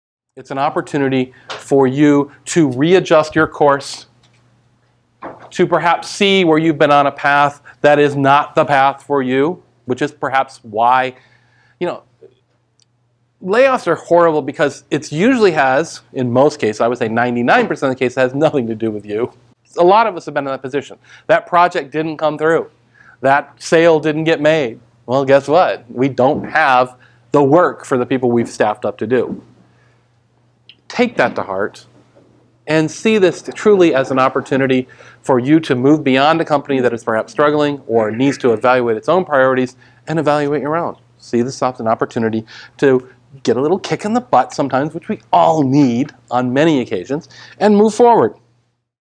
Transition Is Often Not Under Your Control from Transition is the New Normal 2016
Transition is the New Normal 2016

Douglas E. Welch presents "Transition is the New Normal" to OPEN (Outstanding Professionals Employment Network) Ventura County in Simi Valley, CA on March 4, 2016.

Transcript:

It's an opportunity for you to readjust your course. To perhaps see where you've been on a path that is not the path for you. Which is perhaps why -- layoffs are horrible because it usually has -- in most cases -- I would say 99% of the cases -- it has nothing to do with you. A lot of us have been in that position. That project didn't come through. That sale didn't get made. Well, guess, what? We don't have the work for the people we've staffed up to do. Take that to heart and see this truly as an opportunity for you to move beyond a company that is perhaps struggling or needs to evaluate its own priorities and evaluate your own. See this as an opportunity to get a little "kick in the butt" sometimes which we all need on many occasions and more forward.

Also mentioned in this talk is "Career Compass: Finding Your Career North", an Amazon Kindle booklet available

*** Help Support Career Opportunities ***

Your likes help to suggest it to more YouTube viewers.